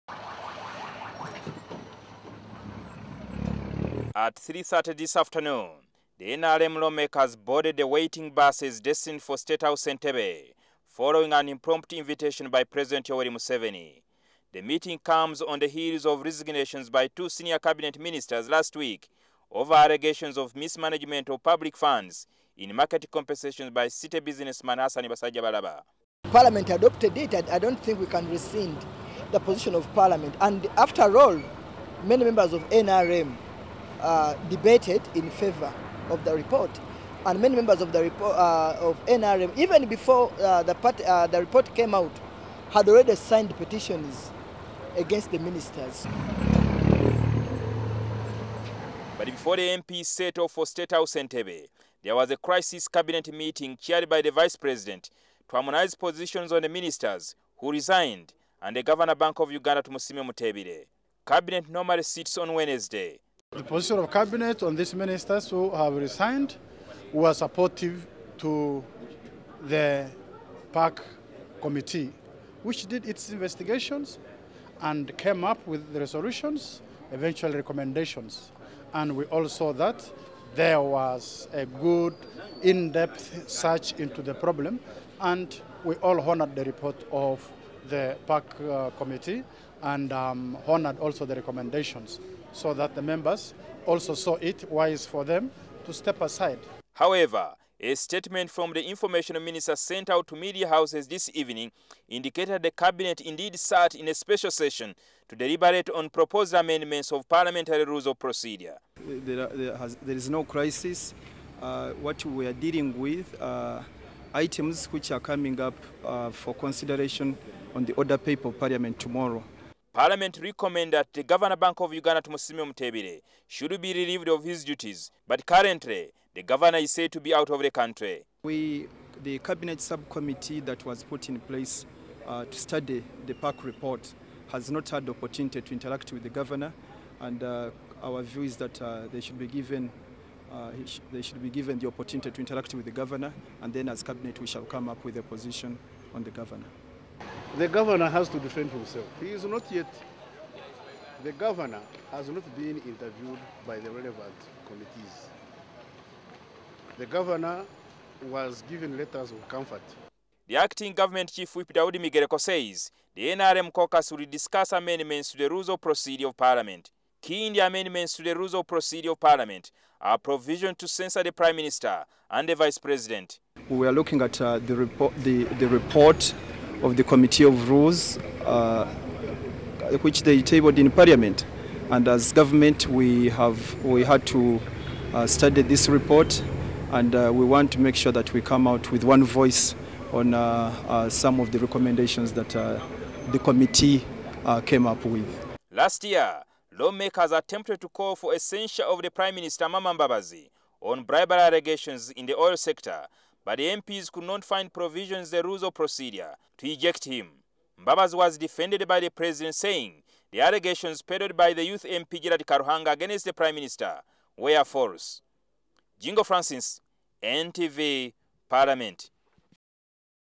President chairs crucial Caucus meeting
NRM Lawmakers are at this hour still holed up at State House Entebbe on invitation by president Yoweri Museveni to discuss several key issues affecting the nation. Legislators believe key on the agenda will be the latest resignations by Ministers and the fight against corruption. However, acting government chief-whip Daudi Migereko told NTV that among the key items on the agenda for the meeting was to discuss amendments to the rules of procedure of parliament